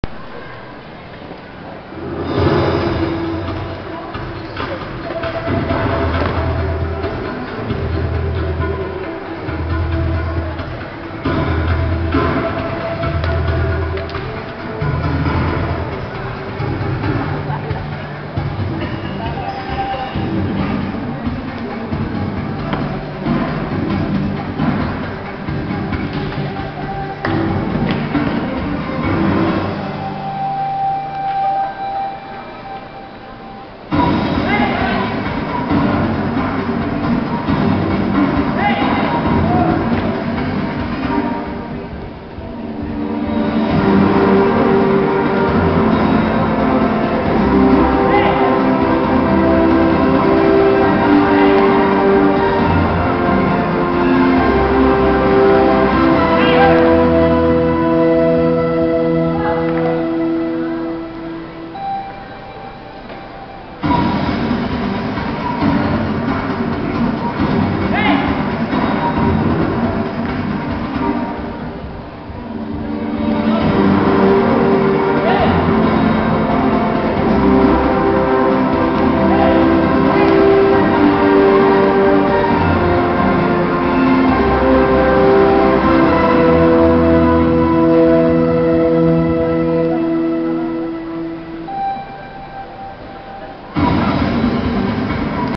GHQ: Bacolod Chapter Adult Synchronize Participant.
Bacolod Tapondo Chapter won gold medal for adult synchronize category in Bacolod City last April 18,2013 during the Regional Tapondo Competition.